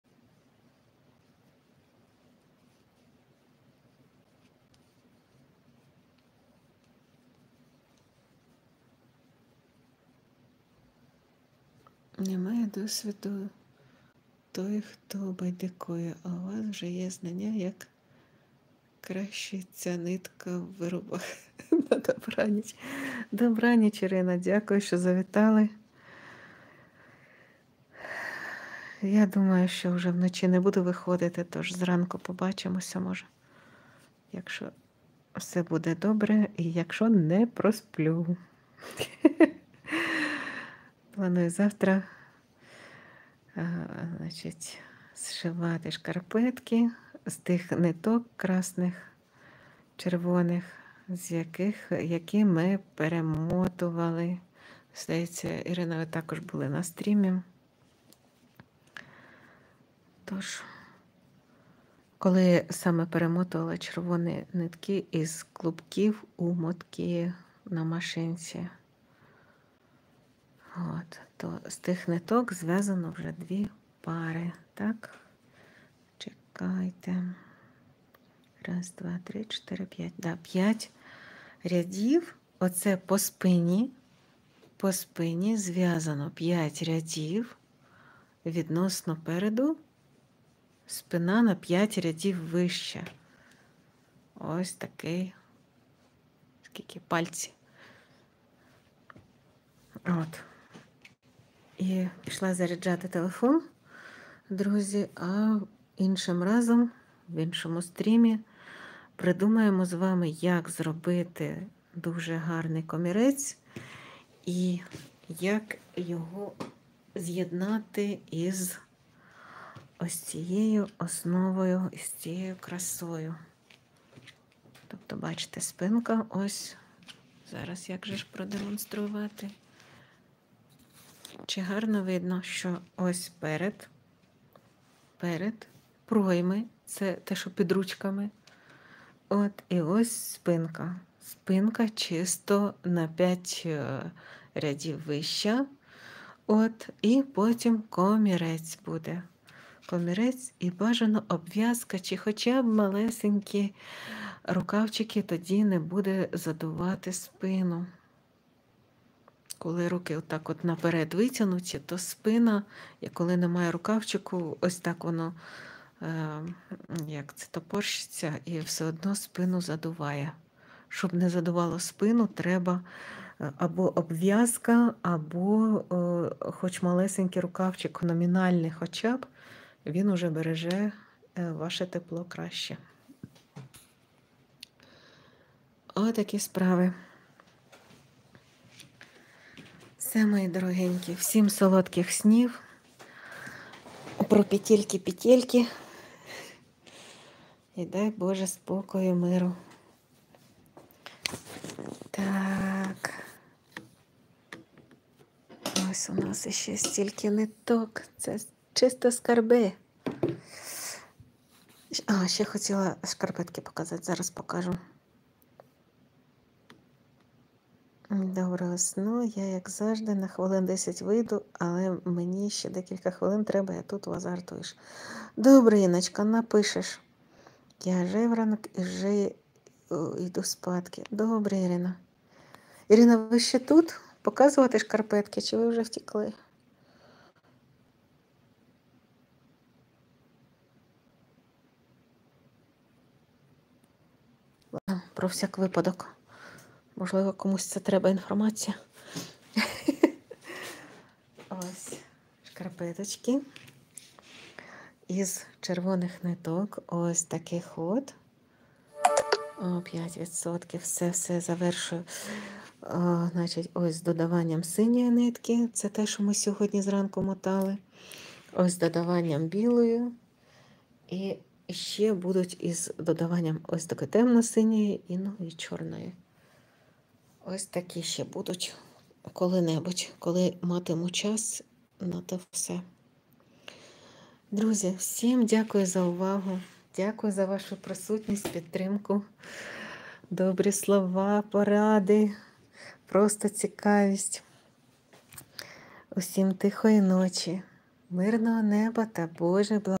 Найкращий момент 2:07:37 – 2:12:37 із трансляції В'язання з Nilena Sans спідниця з Alize Puffy Fine
До вашої уваги майстер класс з в'язання спідниці з Alize Puffy Fine. Дуже просто. впорається навіть дитина.

Рукоділля з Nilena Sans.